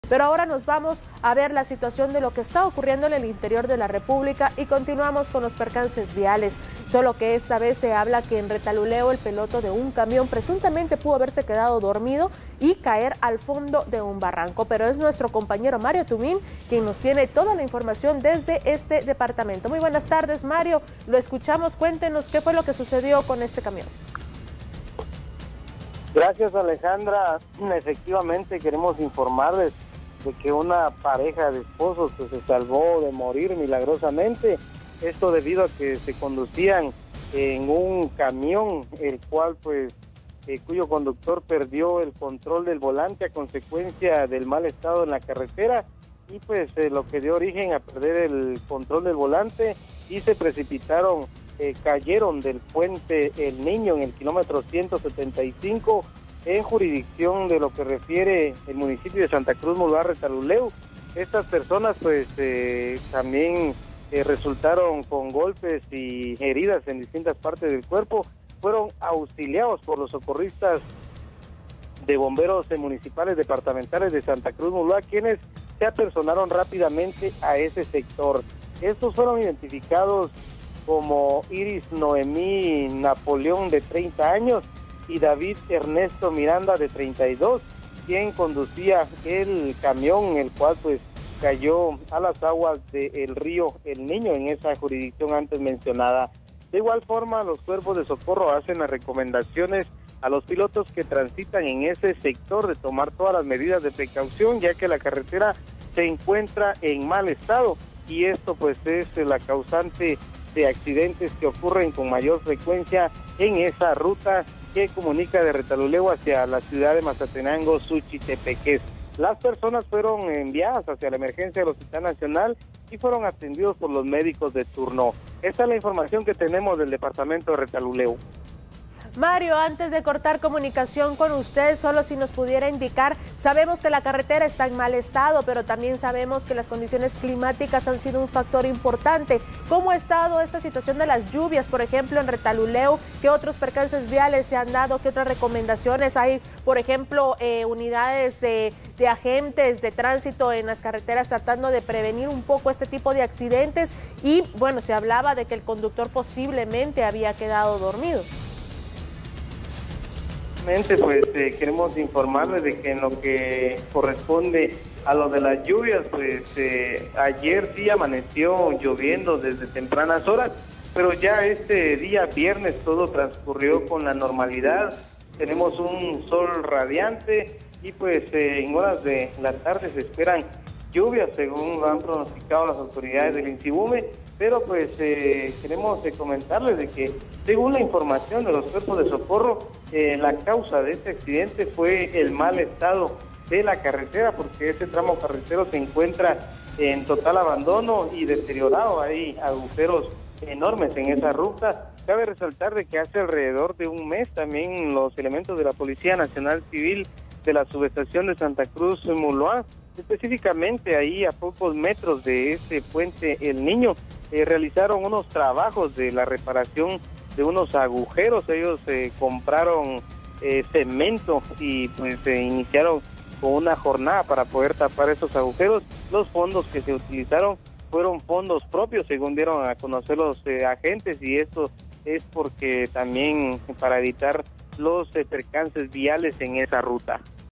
Una pareja cayó al fondo de un río
Se salvaron de morir
El conductor de un camión perdió el control del volante y cayó al fondo de un río en Retalhuleu. Dos personas resultaron heridas por este accidente y fueron trasladadas al Hospital Nacional.